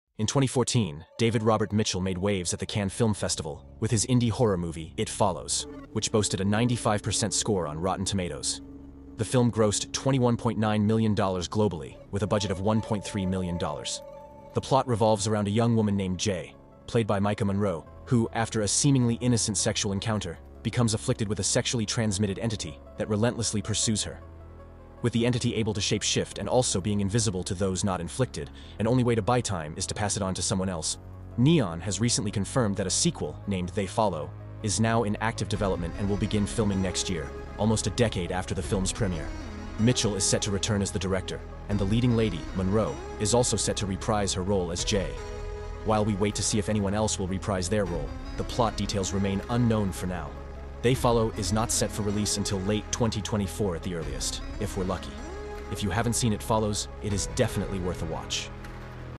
It Follows - Sequel Announced (They Follow)
It Follows is a 2014 American horror film written and directed by David Robert Mitchell. It stars Maika Monroe as a young woman who is pursued by a supernatural entity after a sexual encounter. Keir Gilchrist, Daniel Zovatto, Jake Weary, Olivia Luccardi, and Lili Sepe appear in supporting roles.

It Follows debuted at the 2014 Cannes Film Festival and was later purchased by Radius-TWC for distribution. After a successful limited release, the film had a wide release two weeks later on March 27, 2015.